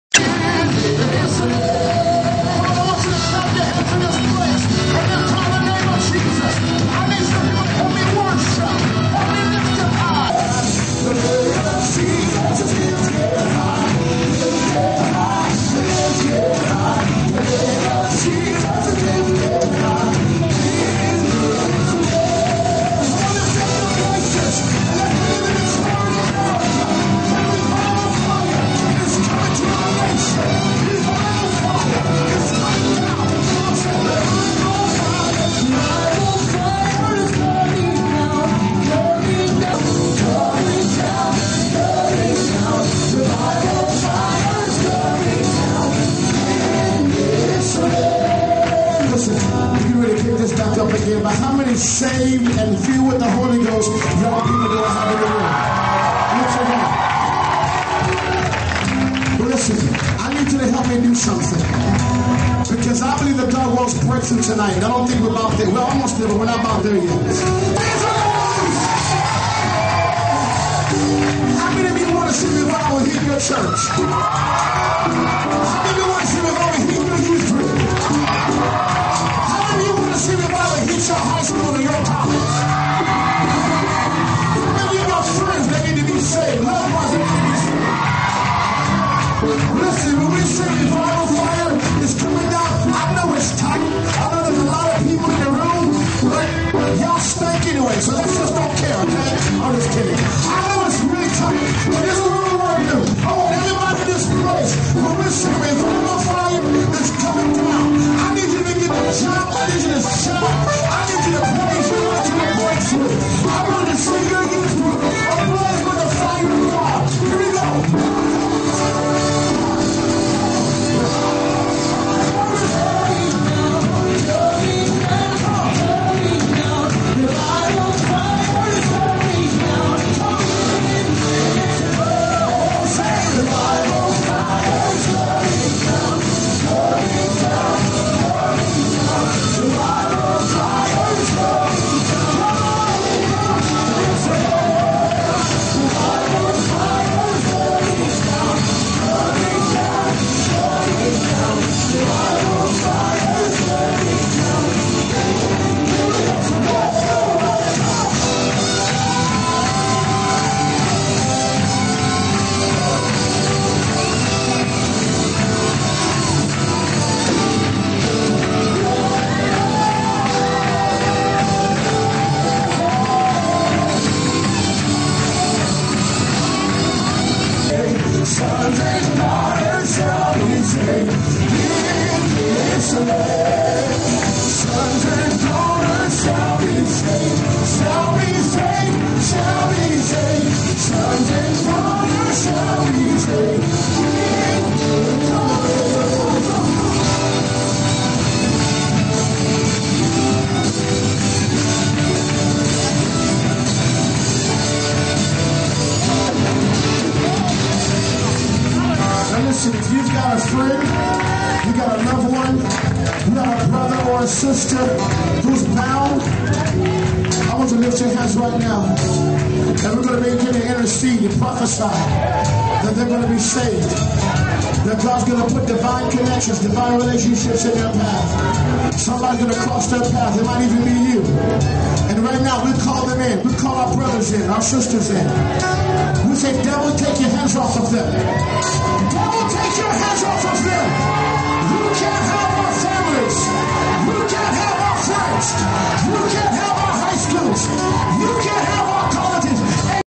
Eddie James Church of God Cathedral
Eddie James at Church of God Cathedral in Winchester, Kentucky 1/12/07